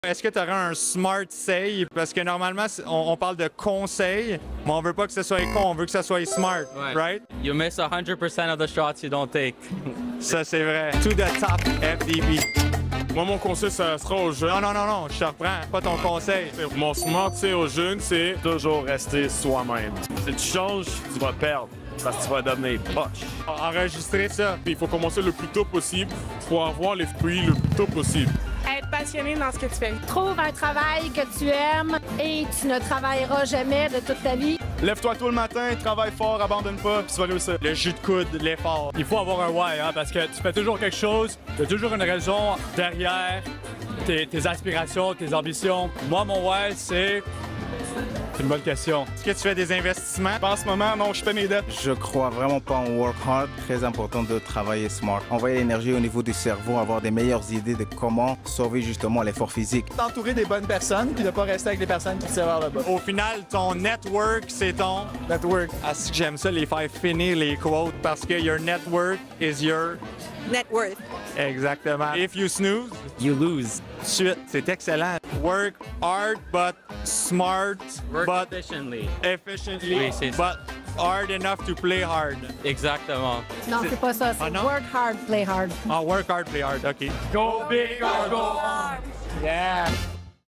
Les smart-seils, pas les con-seils | Comment avoir du succès avec FDB
« Normalement on parle de con-seils mais nous on veut pas que ce soit con, on veut que ce soit smart »

François Daigle « The Eagle » Bérubé sonde une fois de plus l'opinion de l'élite de demain sur leurs trucs et astuces pour réussir dans la vie, un jeudi soir au Cathcart.